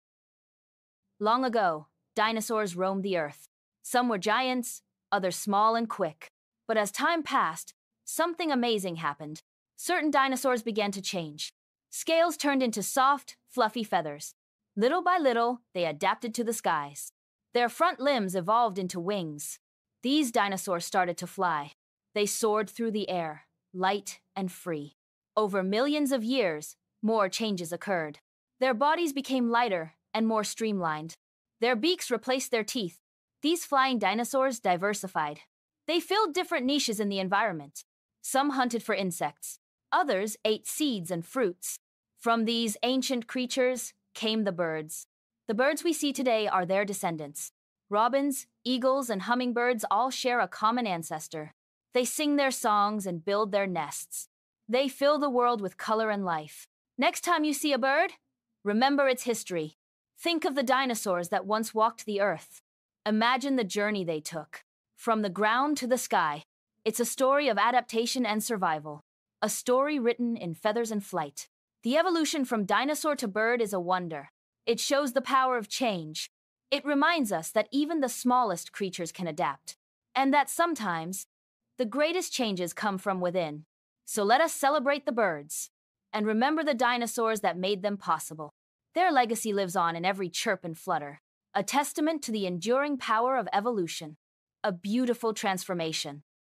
Did Dinosaurs Really Become Birds?
Explore the fascinating link between dinosaurs and birds! Learn about the dinosaur feather and fossil evidence that supports the theory of evolution from ancient creatures. These dinosaur facts will change how you see the prehistoric world.